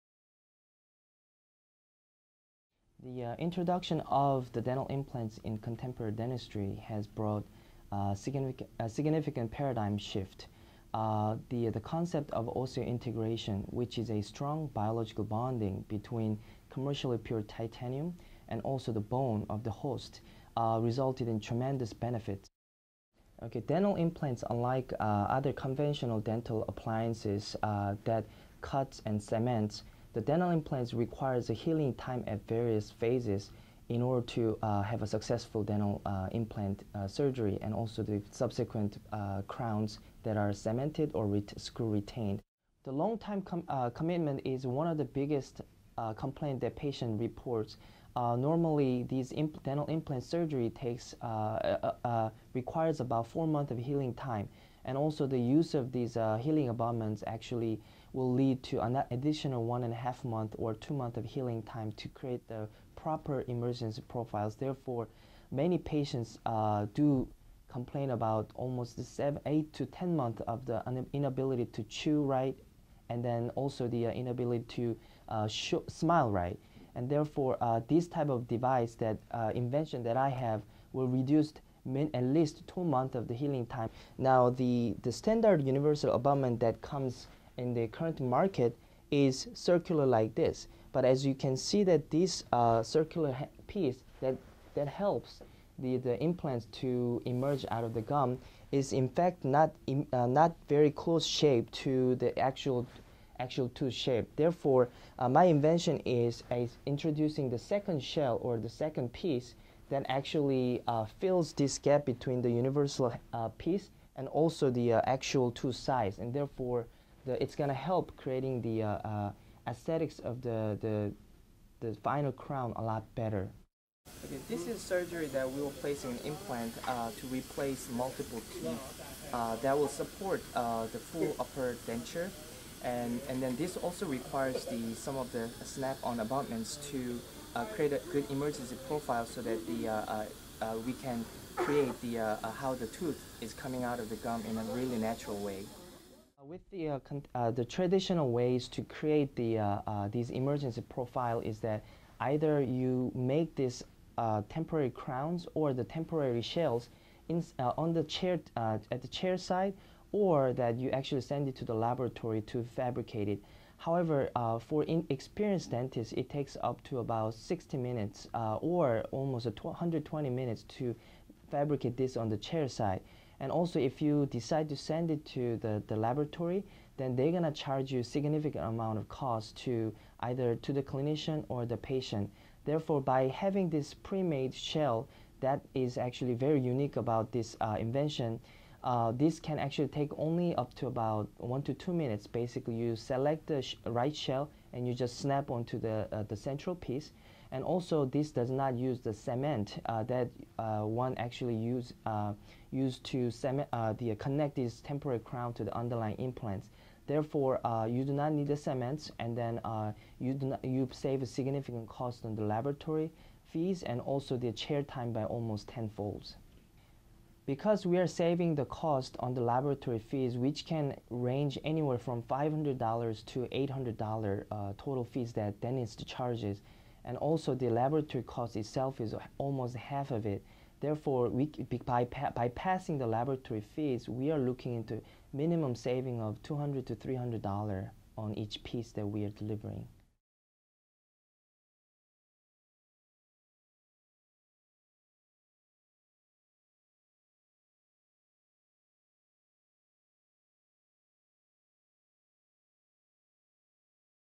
Invention: Snap on Healing Abutment
Dr. Sang Hoon Park, Assistant Professor at the University of Maryland Dental School has developed a low cost solution for dental implant patients that cannot afford temporary crowns during the healing phase of the implant procedure.  Frequently these patients will only have a cylindrical healing abutment in placed in lieu of a temporary crown.  This cylindrical abutment does not allow the patient to chew correctly, is unsightly and creates an unnatural emergence profile.  Dr. Park has created a simple yet elegant alternative to costly healing abutments or the healing abutment alone.